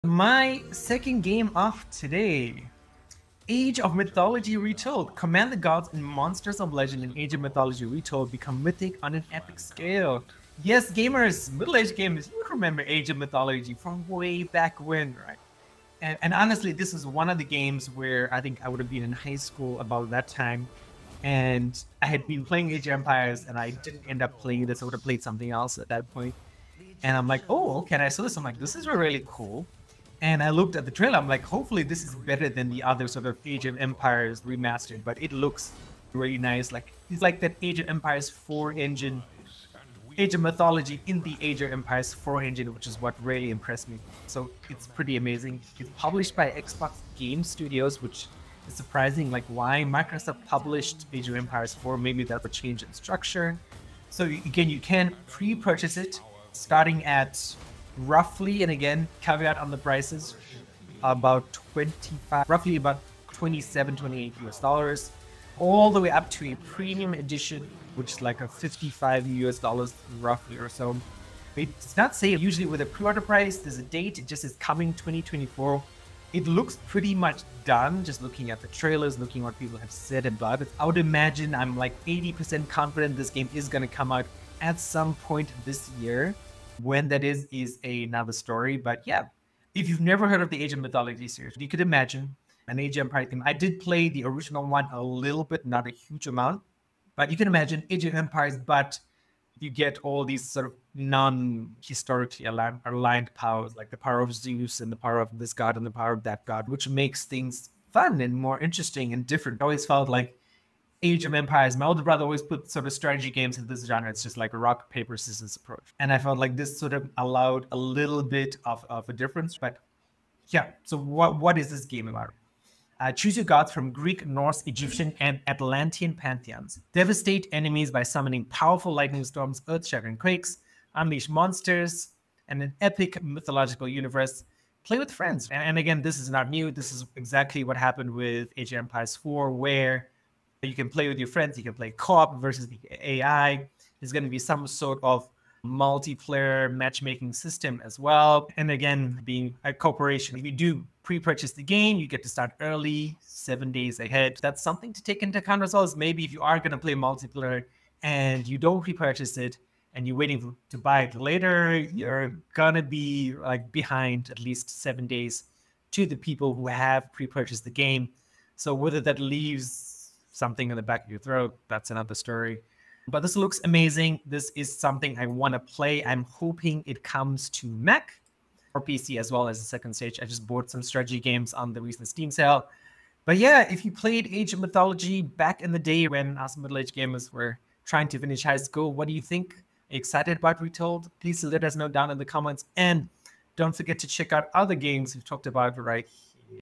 Age of Mythology Retold: A Mythic Return
Dive into the epic world of Age of Mythology Retold, where gods and monsters clash in legendary battles. Discover what's new in this highly-anticipated revival and why it's capturing the hearts of gamers everywhere.

𝙇𝙤𝙫𝙚 𝙊𝙪𝙧 𝙎𝙤𝙪𝙣𝙙 - 𝘽𝙪𝙮 𝙊𝙪𝙧 𝙈𝙞𝙘𝙨: